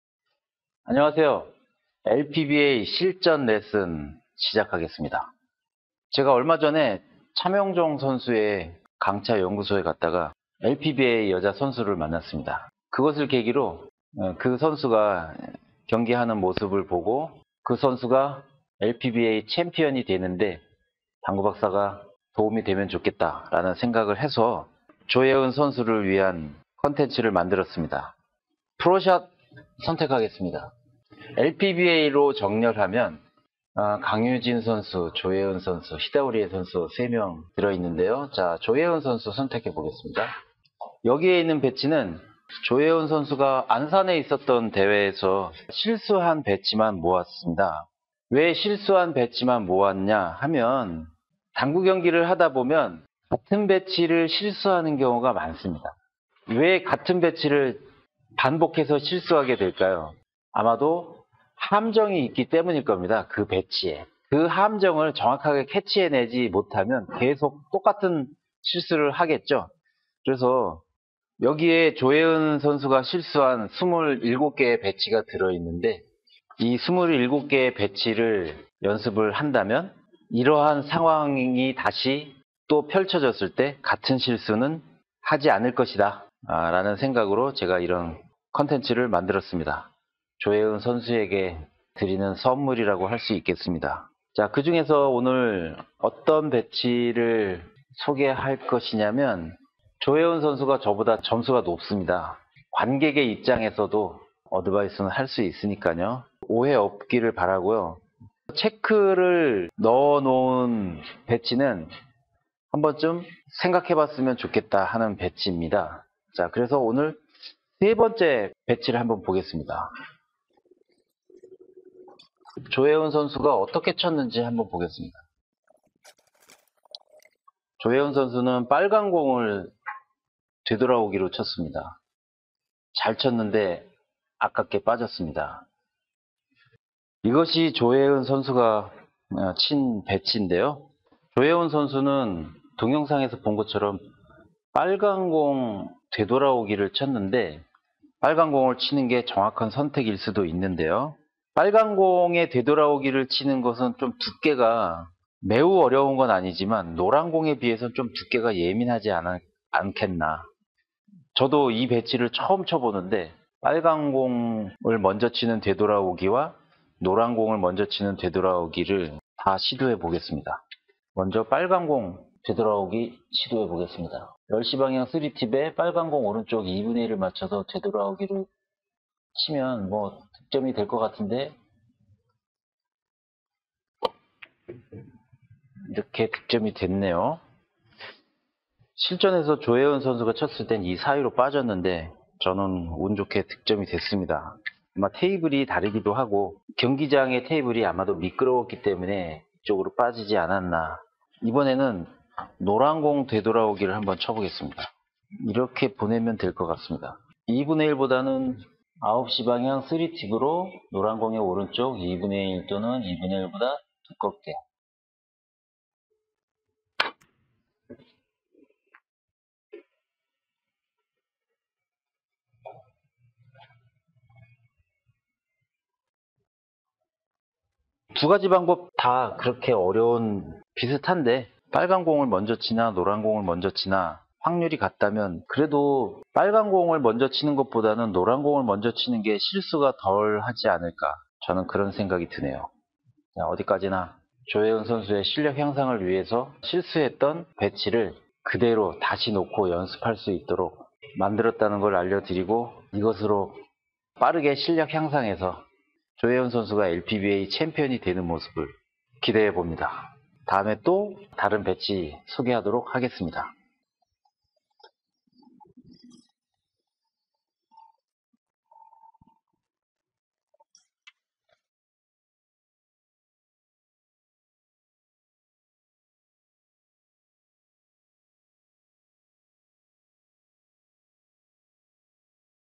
LPBA 실전 당구레슨 - 되돌아오기 1목적구의 선택 (feat.조예은)
LPBA 선수들이 실전에서 친 배치를 가지고 당구 3쿠션 연습을 해보는 내용입니다. 당구박사의 BPS매니저에만 수록되어 있습니다.

당구박사 당구레슨 마스터 (Dr.Billiard's lesson master)